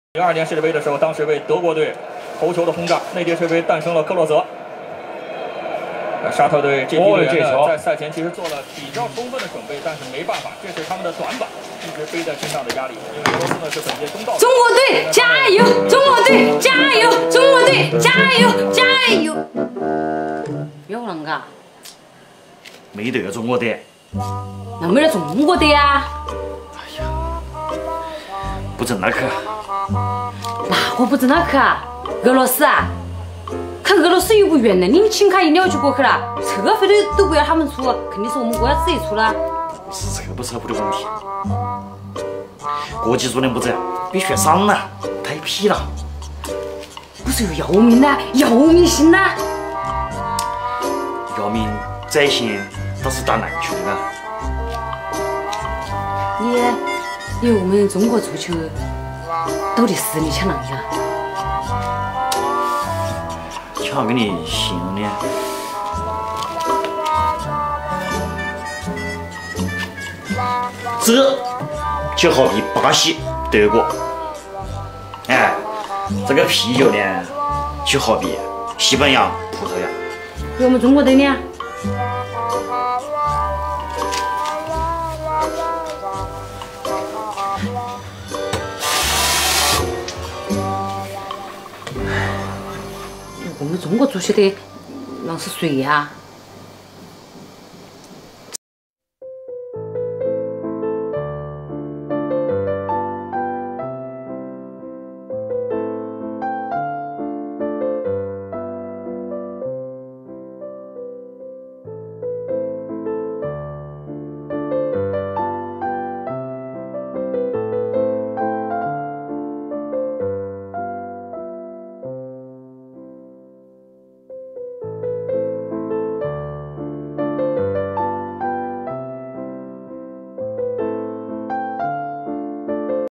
湖北方言喜剧《国足加油》梦想还是要有的，万一实现了呢？
爆笑三江锅致力于原创搞笑视频的创作。 我们强调了视频内容的合规性，绝对没有任何不良引导或危险行为。